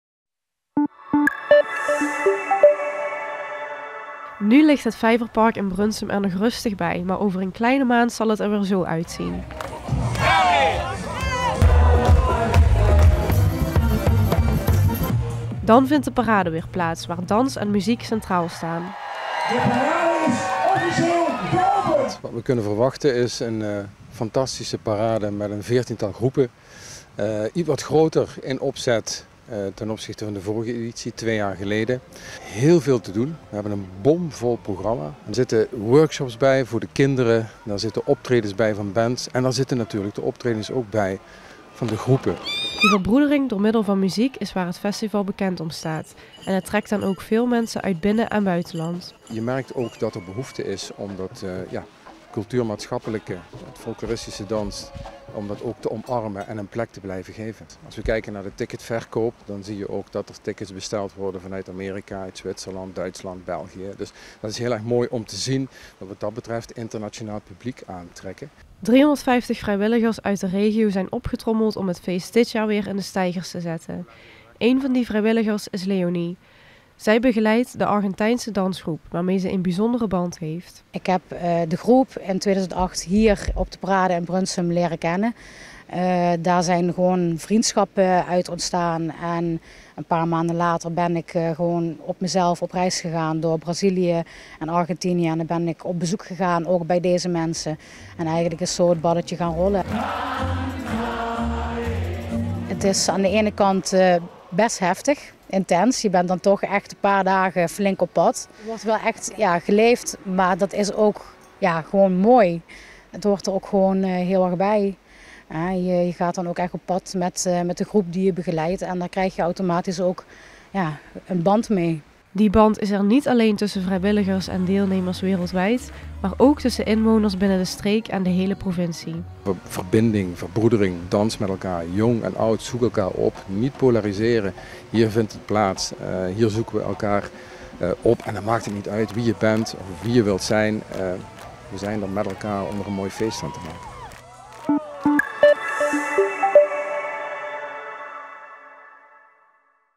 Parade Brunssum verbindt inwoners én de wereld 🌎 | ZO-NWS
Worldfestival Parade Brunssum vindt over een kleine maand weer plaats in en nabij het Vijverpark in Brunssum. Mensen van over de hele wereld vieren door middel van dans en muziek culturele diversiteit tijdens het folklorefestival van 4 juli en met 7 juli. De voorbereidingen zijn in volle gang.

"Het wordt nog groter ten opzichte van de vorige editie", verklapt voorzitter Ron Kamerman. "We hebben een bomvol programma."